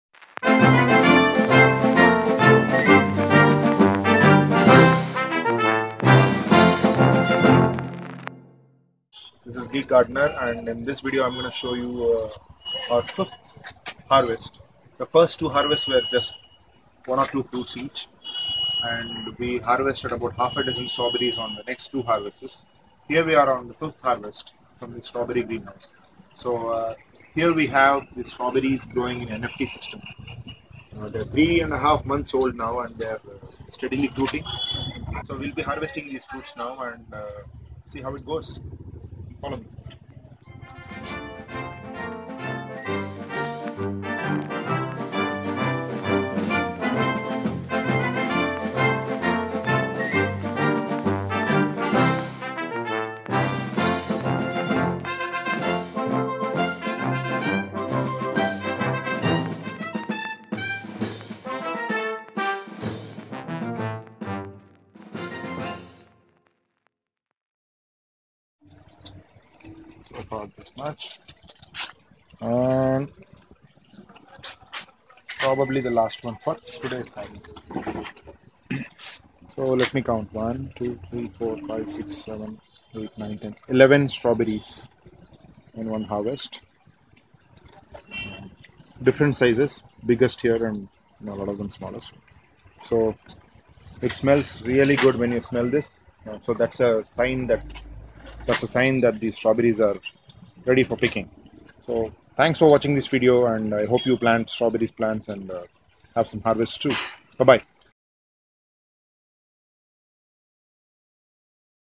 Growing Strawberries - Harvesting Strawberries grown in Nutrient Film Technique
Growing strawberries is fun and easy if you follow these tips. Learn how to grow strawberries hydroponically in this video. You will be able to create your own strawberry patch with these tips on growing strawberries.